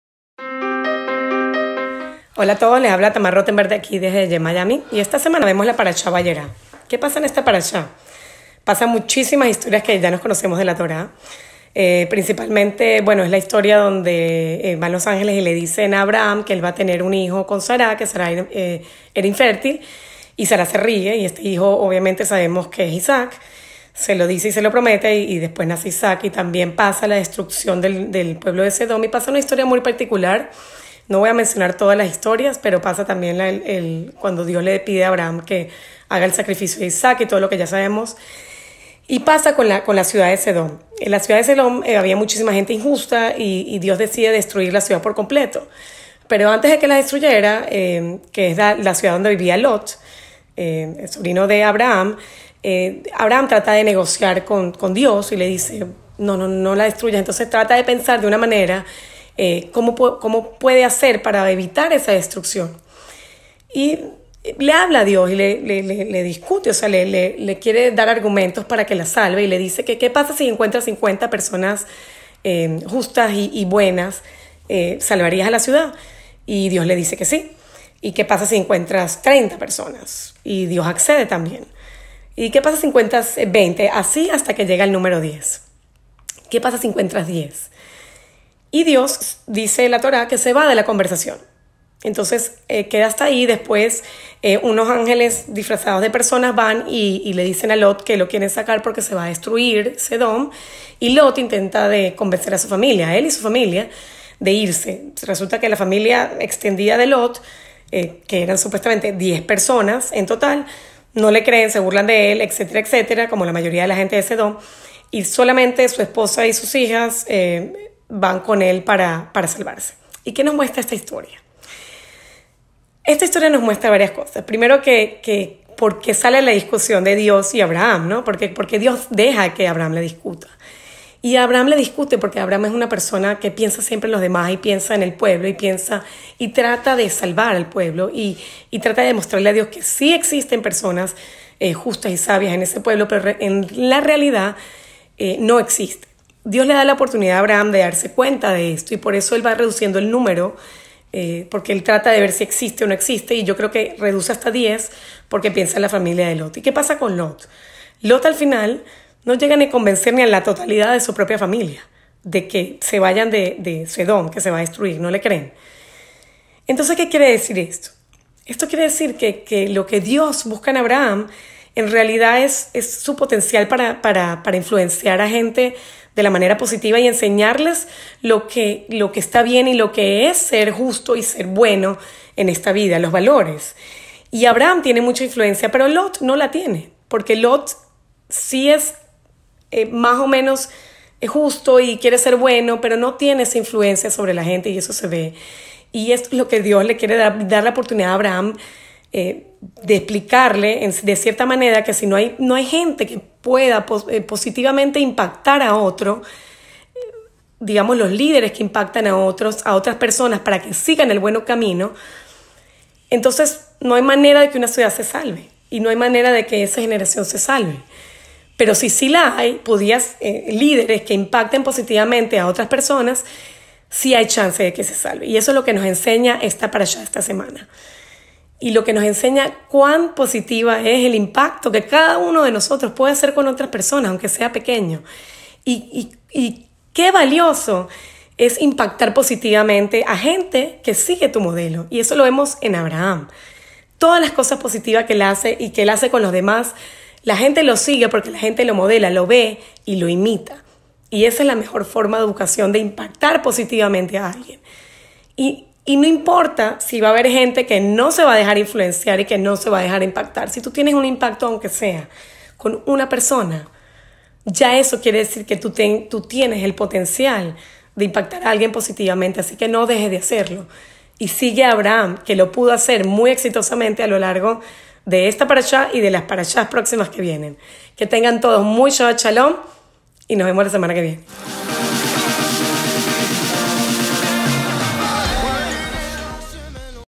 Parasha Vayera / Jem Miami
Reflexiones sobre la parasha Vayera . Tengan un impacto positivo en la gente aprendamos de abraham